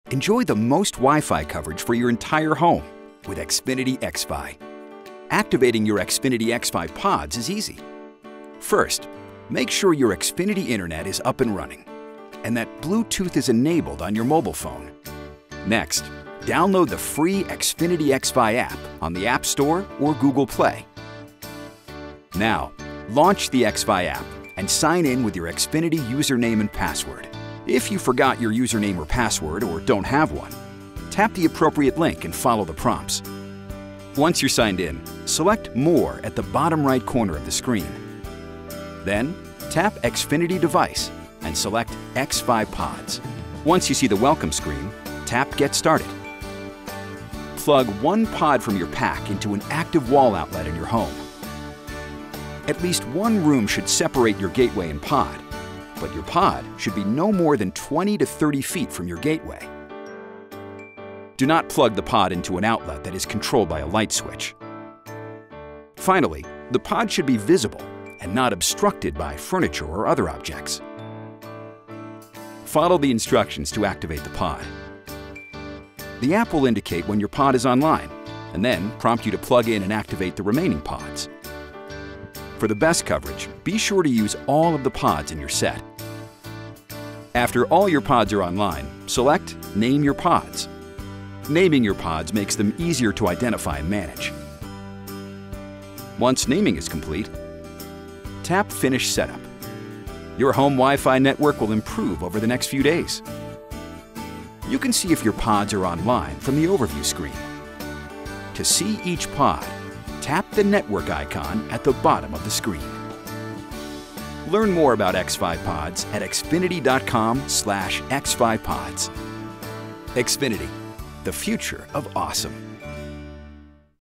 Xfinity xFi Pods Installation & Activation
Installing your xFi pods is easy with the free Xfinity xFi app. This video covers installation, activation and naming your pods.  It’s never been easier to get the most coverage and control for your connected devices.